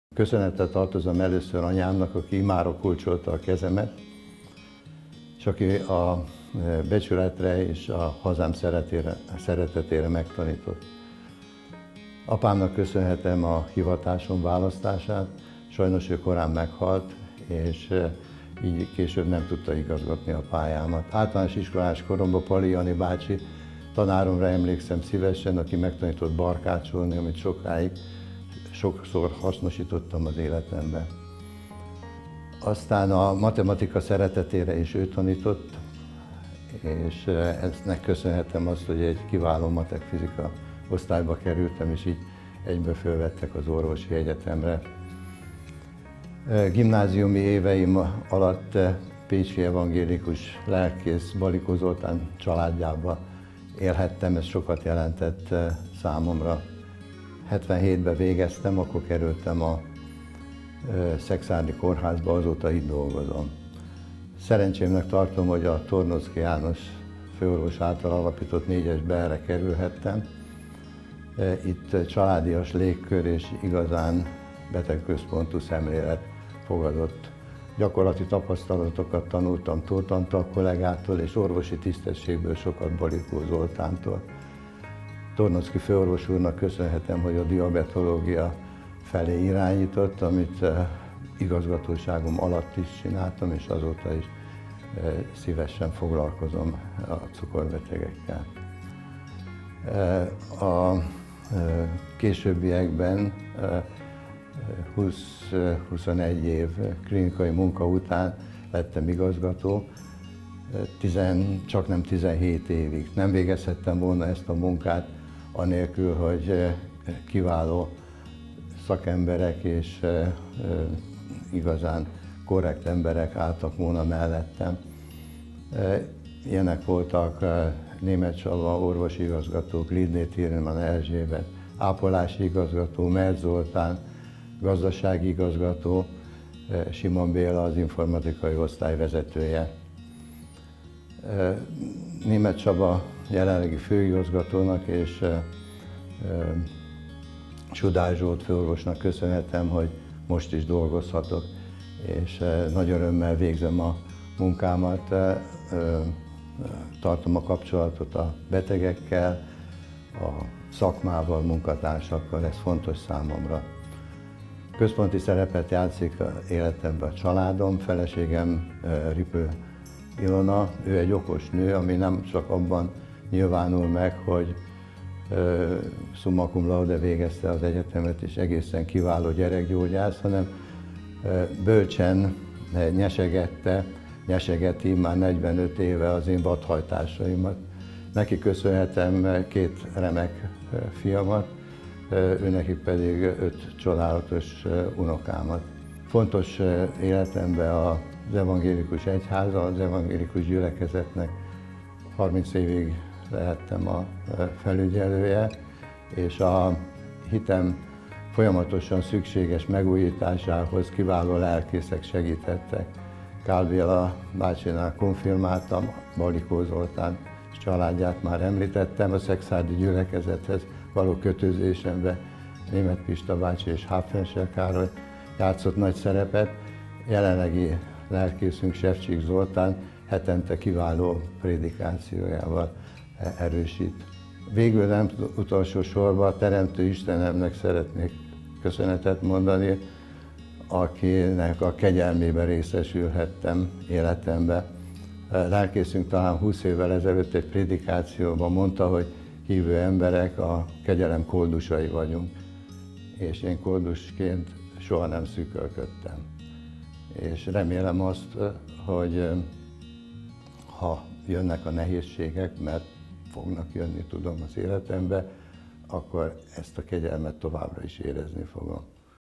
Dr. Muth Lajos, belgyógyász, diabetológus főorvos
Dr. Muth Lajos, belgyógyász, diabetológus főorvos 1998 – 2015 között a szekszárdi Balassa János kórház igazgatója